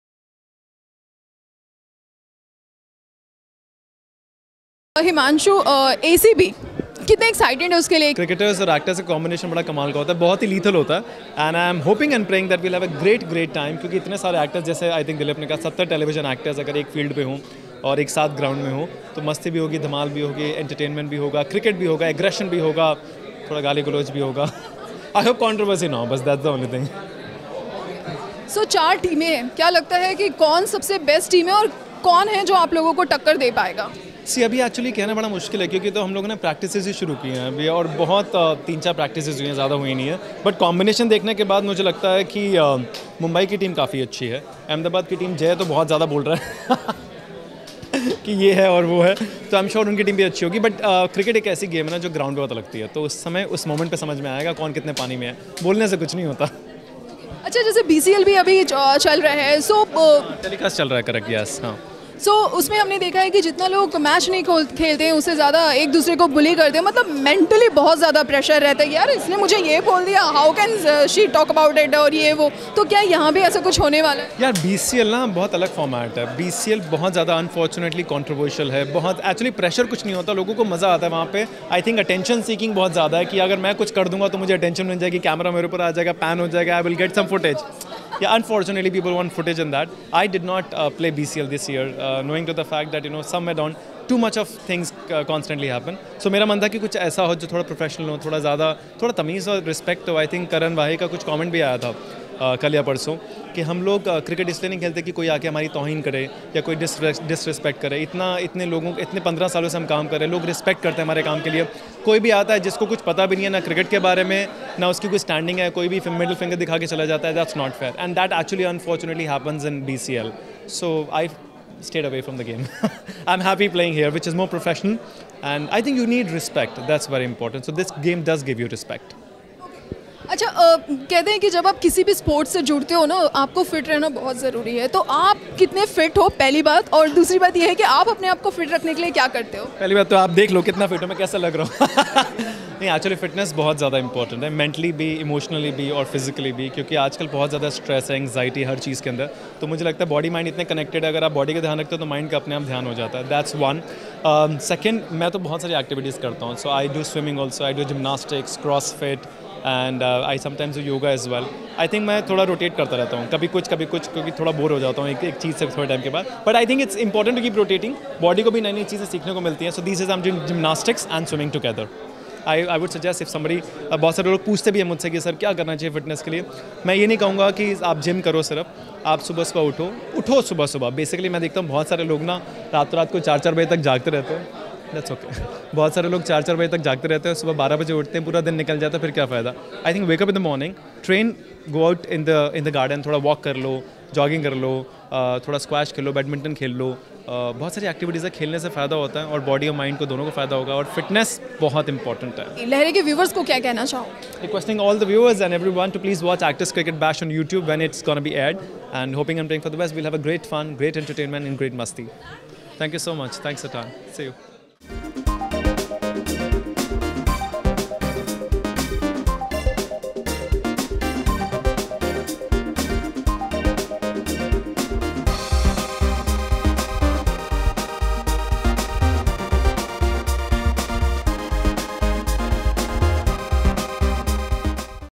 Fitness Mantra With Himanshu Ashok Malhotra | Exclusive Interview
In An Exclusive Interviw Himanshu Ashok Malhotra recently gave a fitness tip how to keep your body building higher.Want To Know More Click This Video.